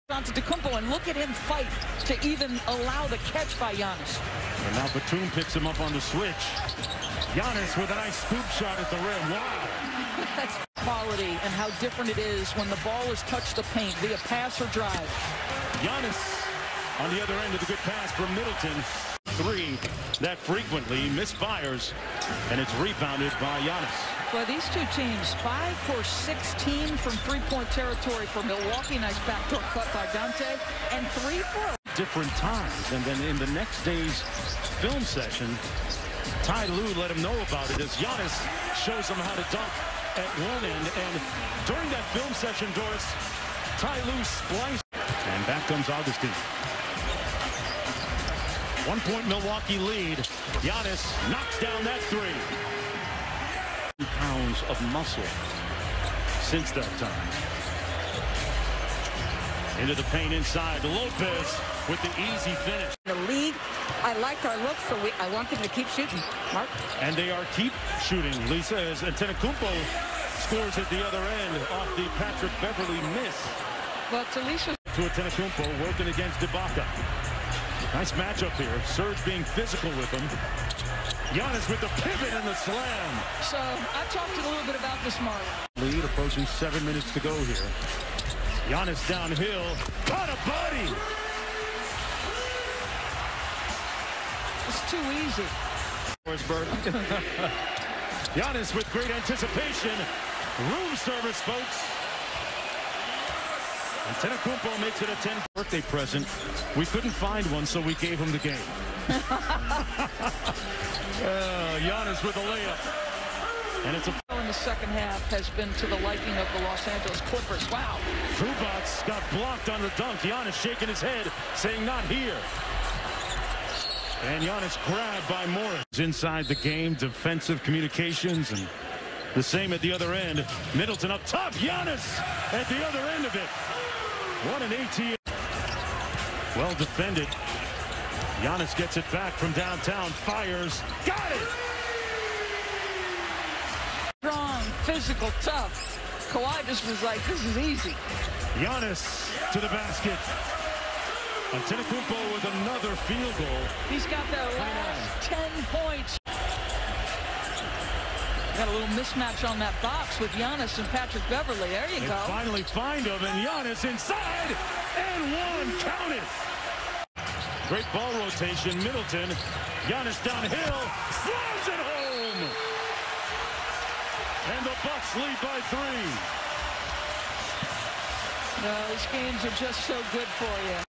Giannis Antetokounmpo 36 Pts, 14 Rebs, 4 Blks vs Clippers | FULL Highlights
Milwaukee Bucks' Giannis Antetokounmpo Highlights vs Clippers - 36 Pts, 14 Rebs, 5 Asts, 4 Blks, 1 Stl, 15-27 FG in 40 Minutes